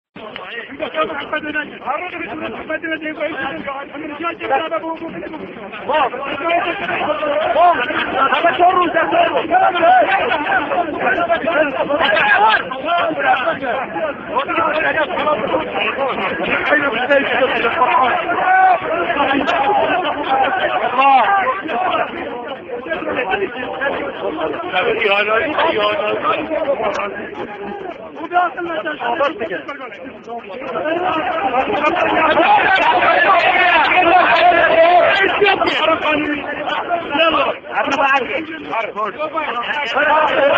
Kurden greifen die Yeziden in Sheikhan an (9)
Kurdischer Stamm greift die Yeziden
in Sheikhan (Ain Sufni/Welatschech) an

Vorfälle in Sheikhan am 14. und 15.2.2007